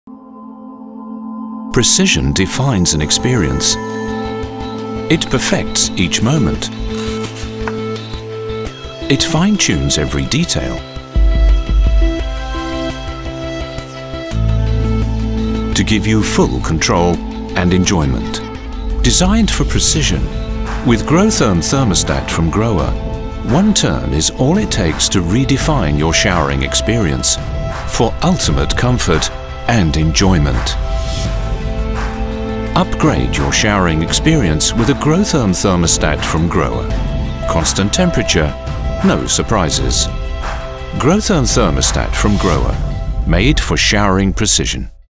Grohtherm Thermostats by Grohe - Indulge in Grohe Luxury Shower
Enjoy a showering experience with utmost safety and comfort for you and your family. Chose Grohe Thermostat for the redefined way of showering. It has Grohe TurboStat - delivers safety in precision through the right mix of hot and cold water and Grohe Cool Touch - ensures the outer surface of the thermostat is never too hot to touch.